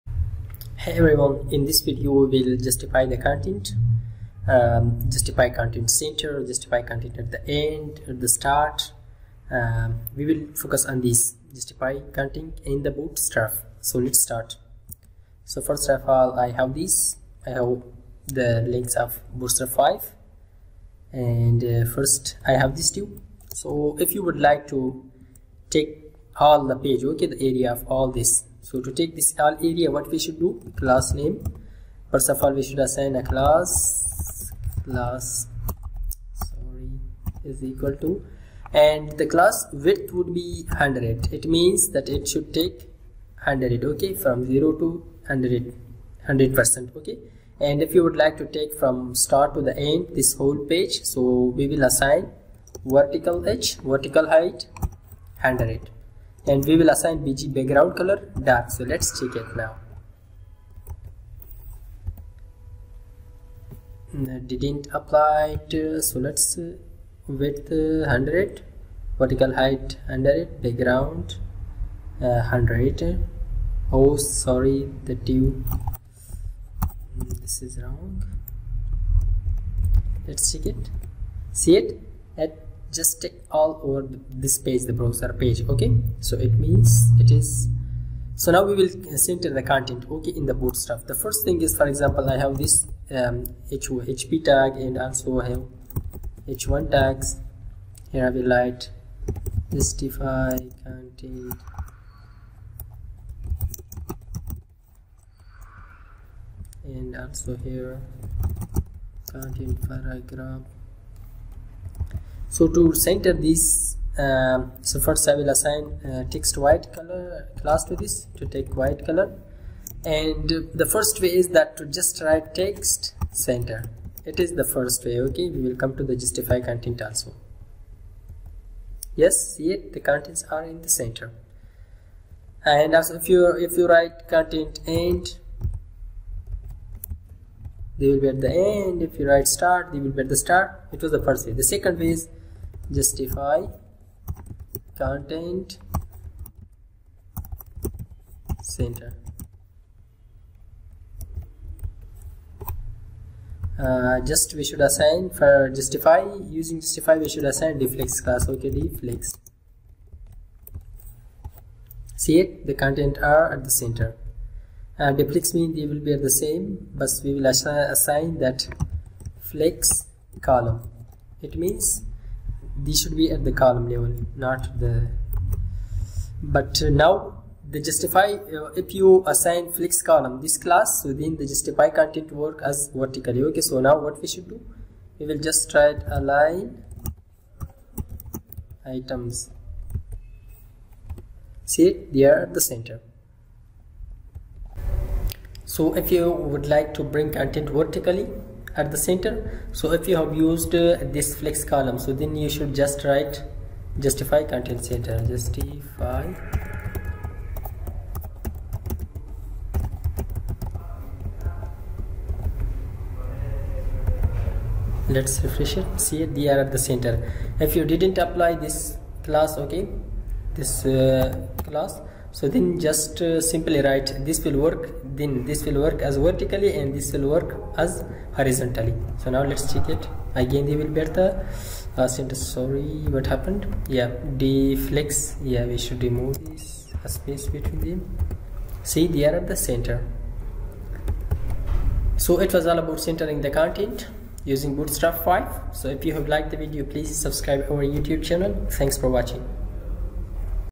Justify Content Bootstrap 5 | Center Content using Bootstrap 5
How to center content horizontally and vertically on a web pages using bootstrap 5. Centering content on a web page with bootstrap 5 can be done in 2 way.